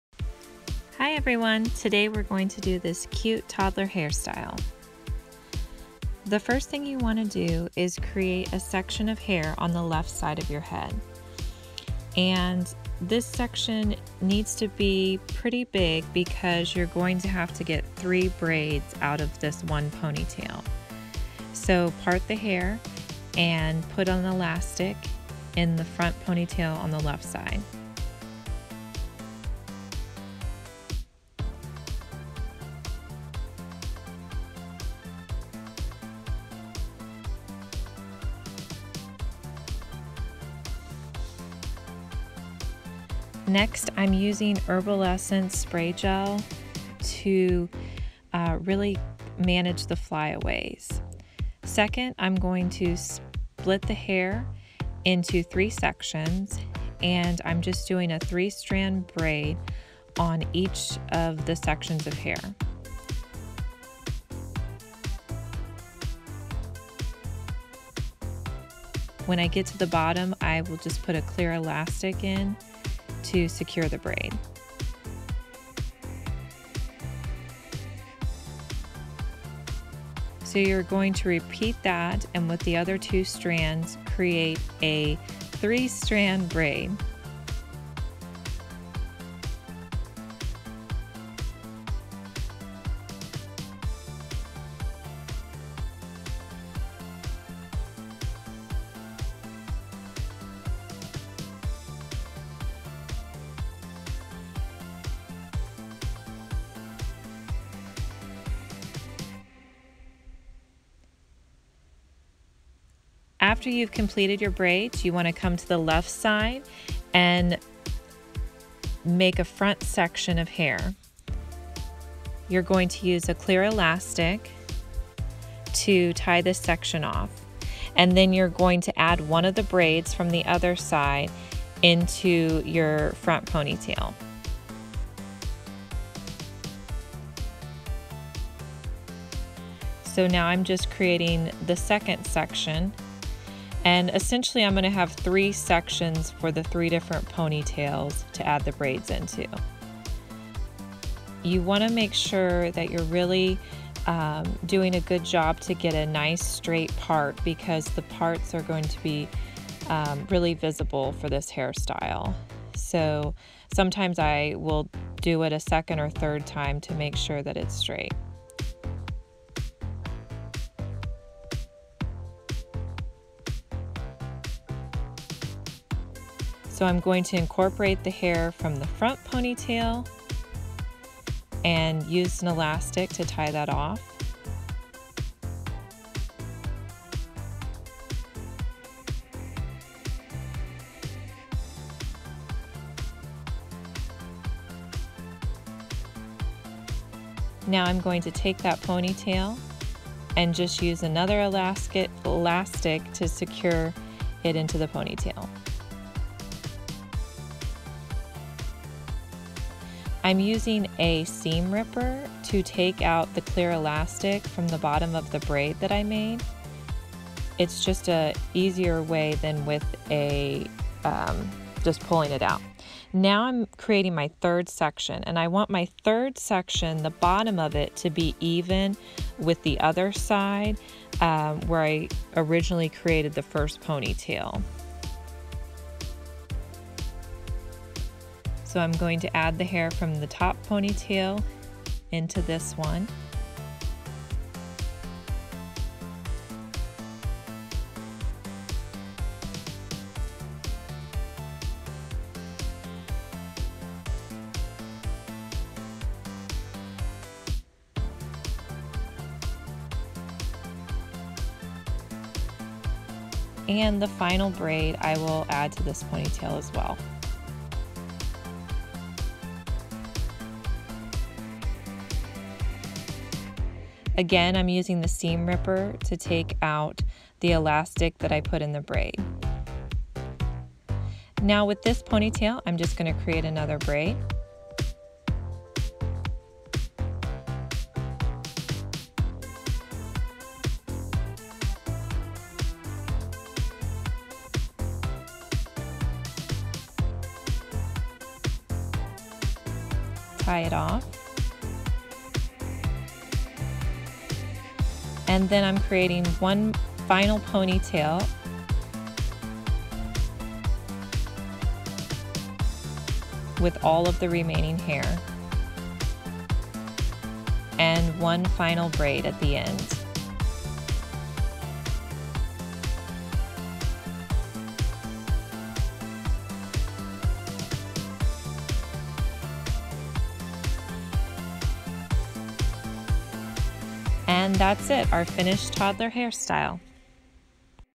How to toddler hairstyles - short hairstyle
This toddler hairstyle is perfect for short hair or fine hair. This style is easy to do and is a cute hairstyle. This style would work on any length of hair. Enjoy!

Products used in this video:
Herbal Essence Spray gel